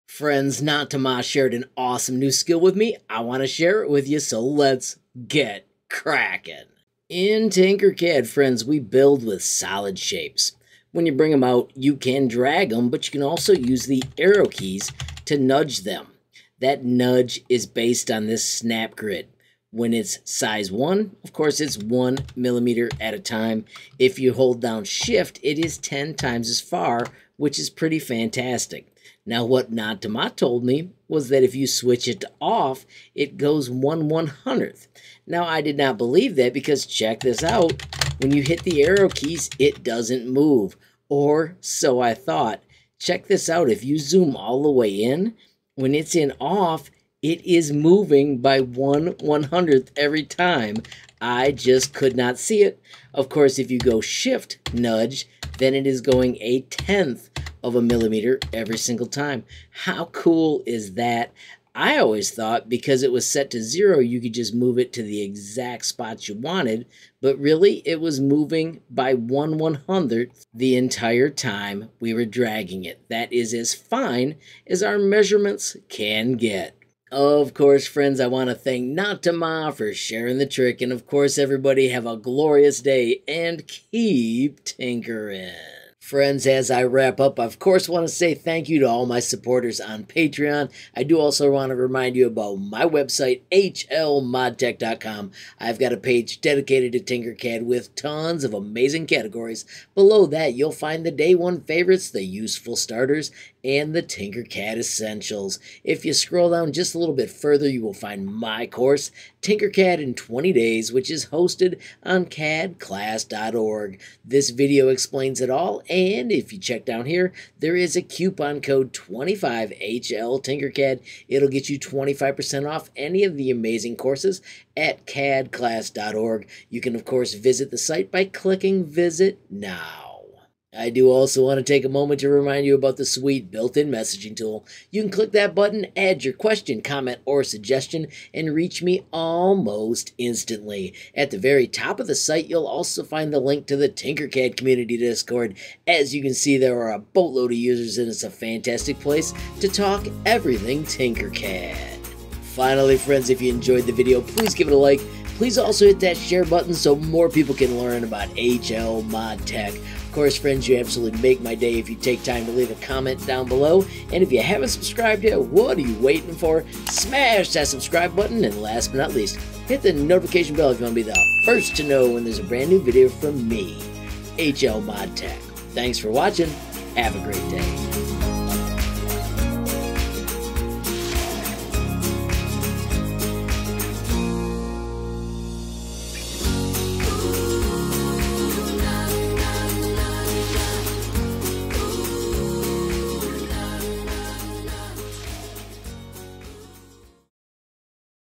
Unlocking The Secrets Of Tinkercad: Mastering Ultra-precise Nudging With Natama
Unlocking The Secrets Of Tinkercad: Mastering Ultra-precise Nudging With Natama. Advanced Tinkercad Hidden Features Ultra Precise Nudge in 3 Minutes! A Super huge shoutout to NaTaMa for the tip. I initially did not believe it, but after testing, I found out I was wrong! Thanks for being persistent NaTaMa! Have a glorious day and keep Tinkering!